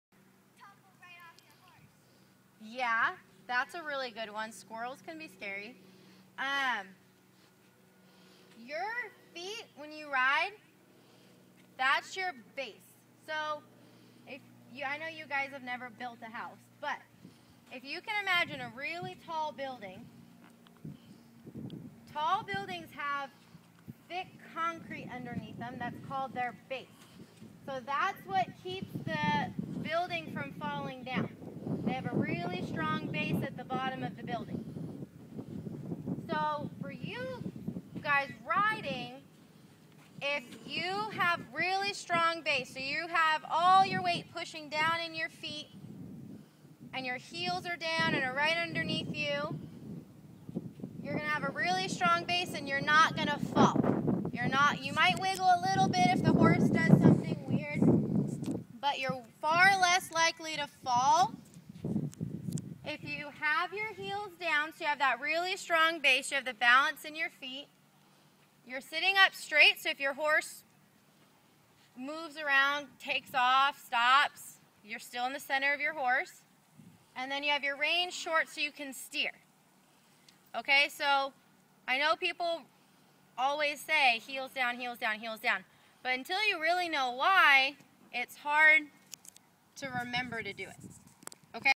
Kelly Taylor teaches horseback riding in Lemoore
Kelly Taylor teaches a class in horseback riding at the Circle T Ranch in Lemoore on Saturday morning. (iPhone video by Gary Feinstein/The Sentinel)